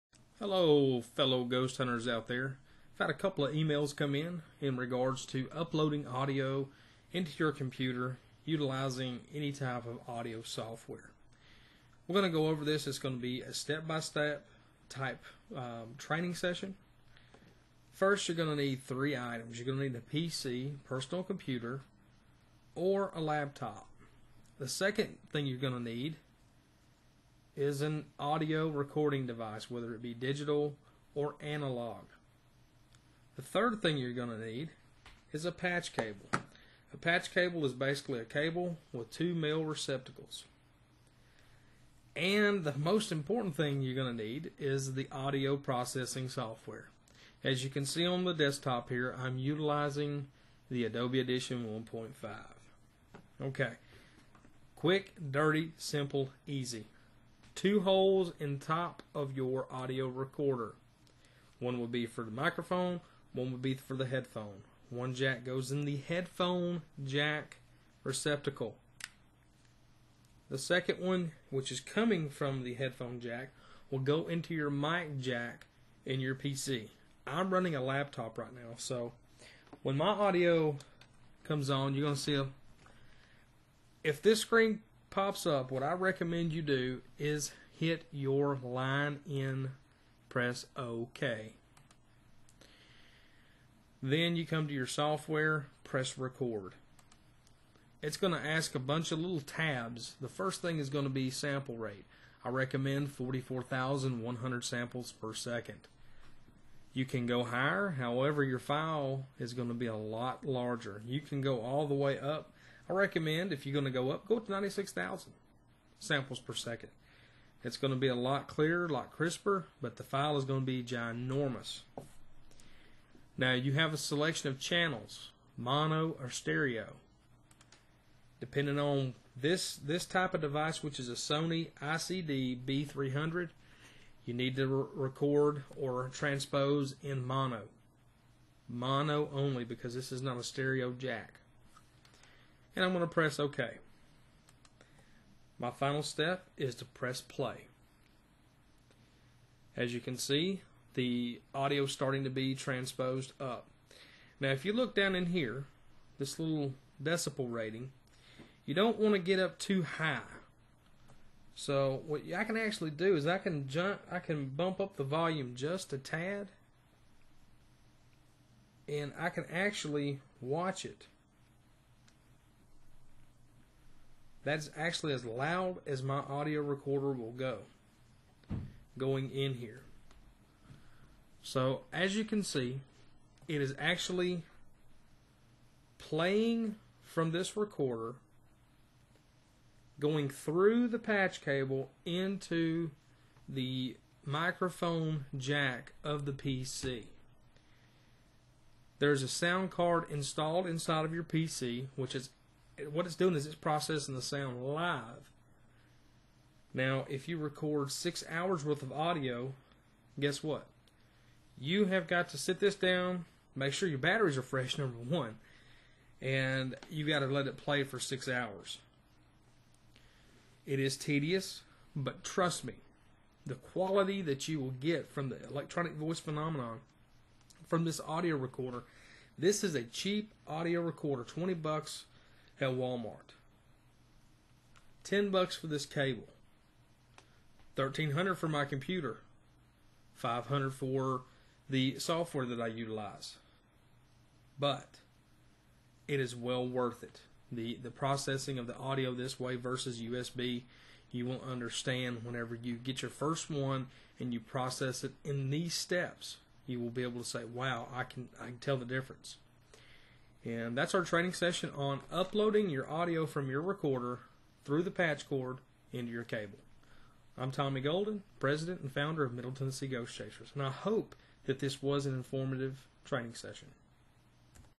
Uploading Audio to Adobe Audition
Uploading Audio to Adobe Audition using a patch cable.